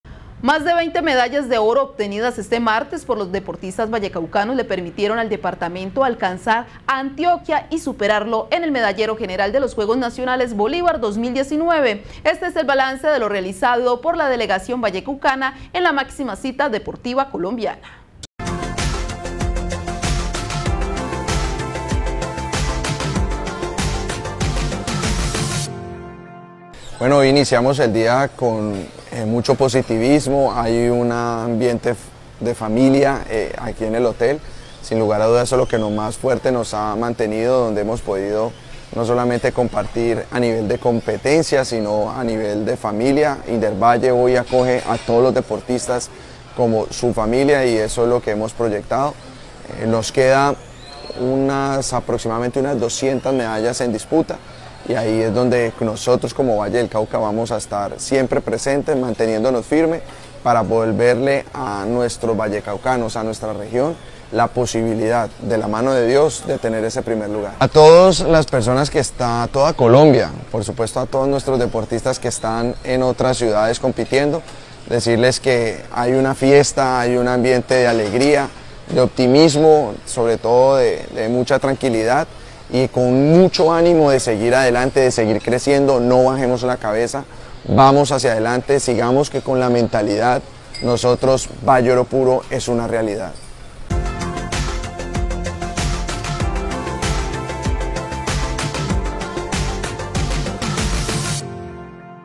Valle, primer lugar en Juegos Nacionales.
Las 23 medallas de oro obtenidas este martes, por los deportistas vallecaucanos, le permitieron al departamento alcanzar a Antioquia y superarlo en el medallero general de los Juegos Nacionales Bolívar 2019. Este es el balance, de lo realizado por la delegación vallecaucana en la máxima cita de los deportes en Colombia.